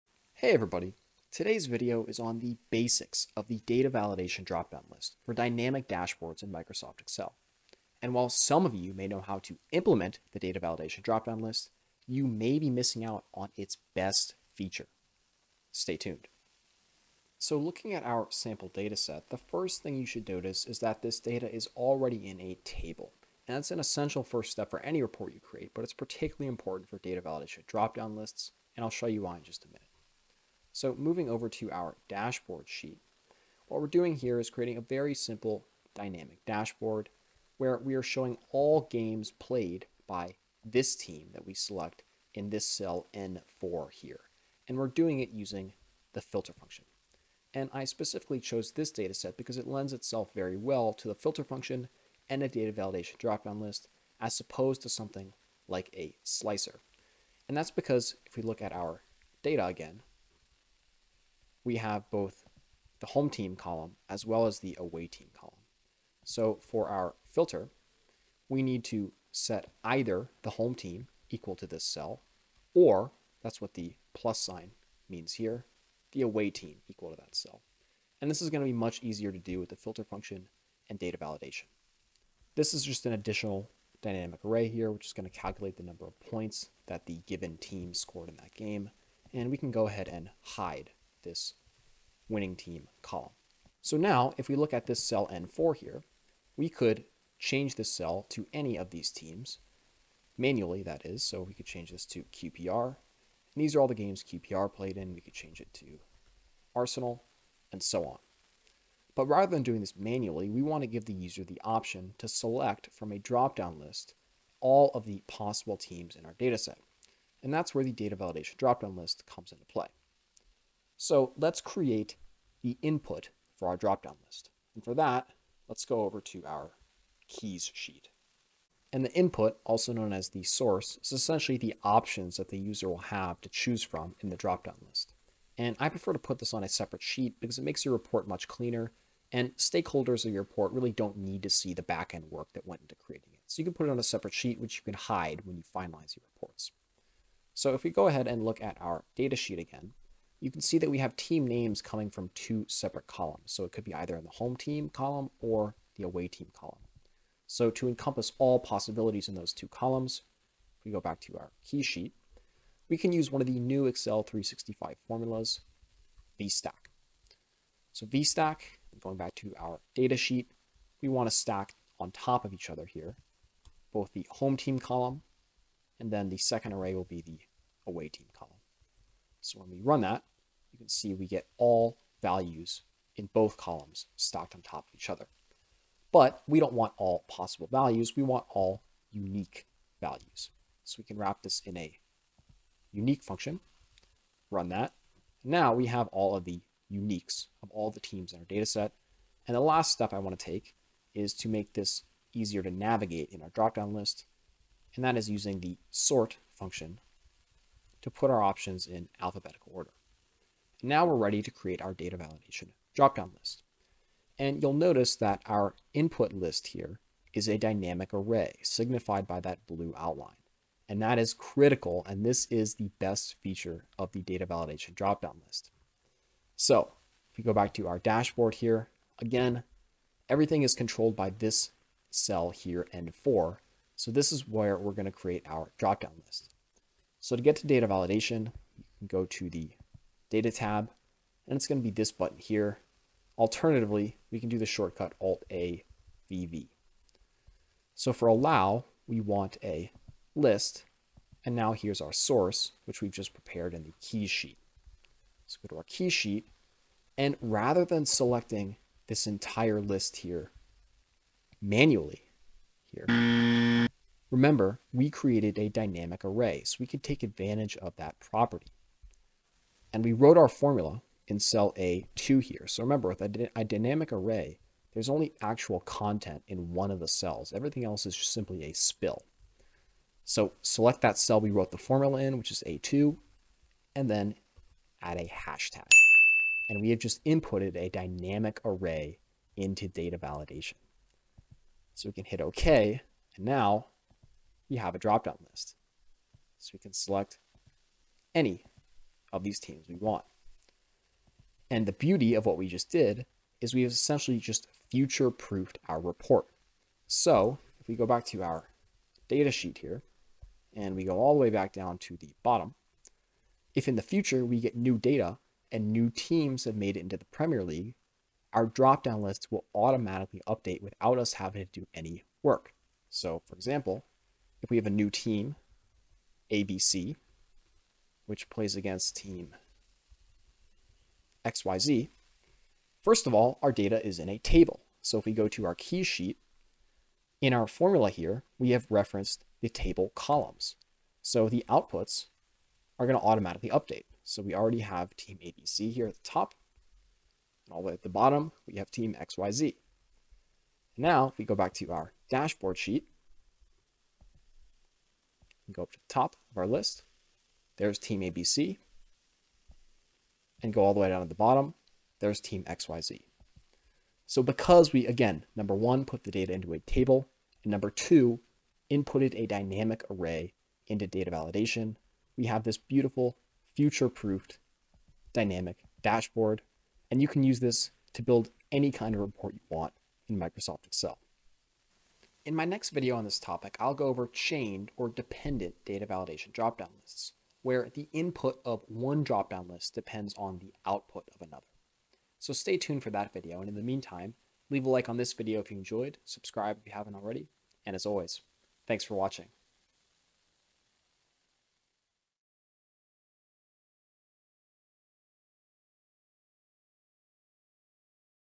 Data Validation Dropdown Lists in Excel (the basics)
This video teaches the basics of the data validation dropdown list, which is an extremely useful tool for creating dynamic dashboards in Microsoft Excel. While dropdown are simple to implement, they must be created correctly -- using data from tables and dynamic array inputs. These simple additions to your dropdown list will future-proof your work -- meaning that your report will update automatically with new data without any work on your part.